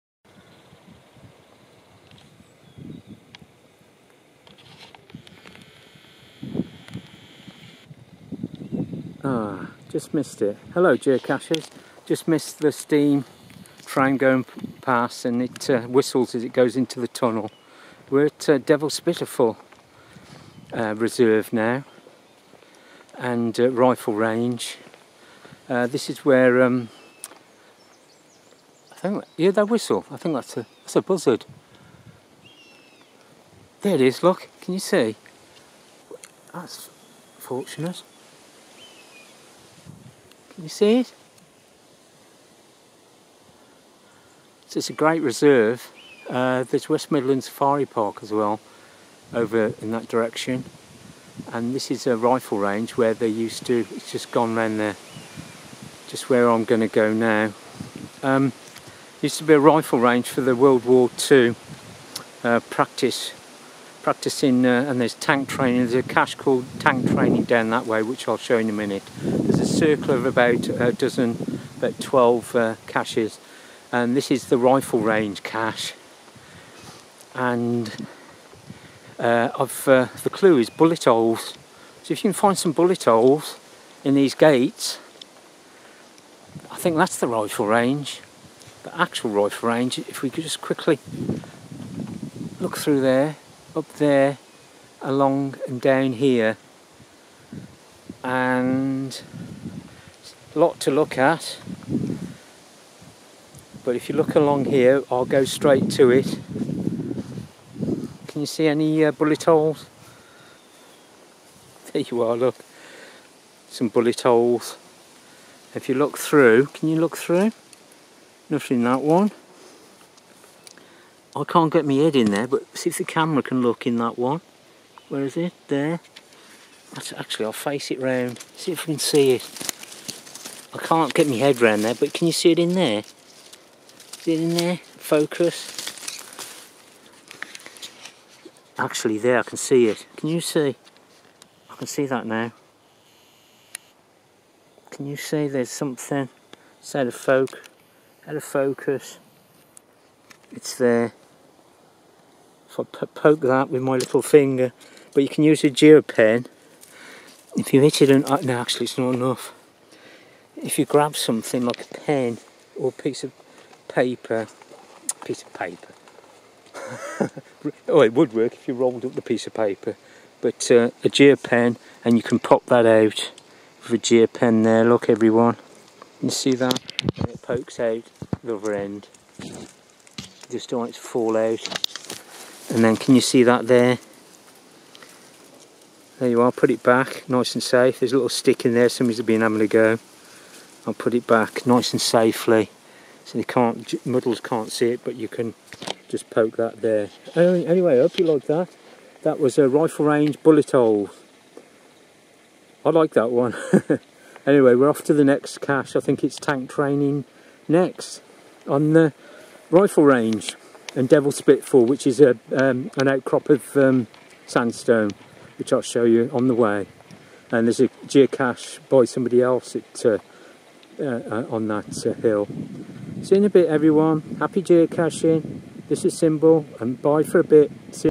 Rifle Range bullet holes cache ! Geocaching Adventure Vlogger UK
The Worcestershire Wildlife Trust reserve was used in the second World War for target practice and Tank training... it was also a camp for the US Army Hospital at Burlish Top ! Stourport Bewdley

Craft caches & Geocaching adventures - Simball Geocache Vlog UK